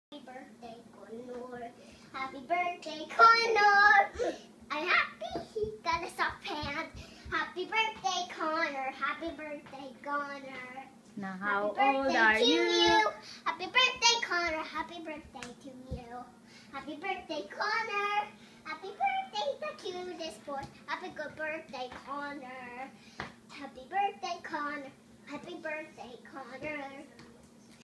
Connor's 1st birthday 2015
His sister singing to him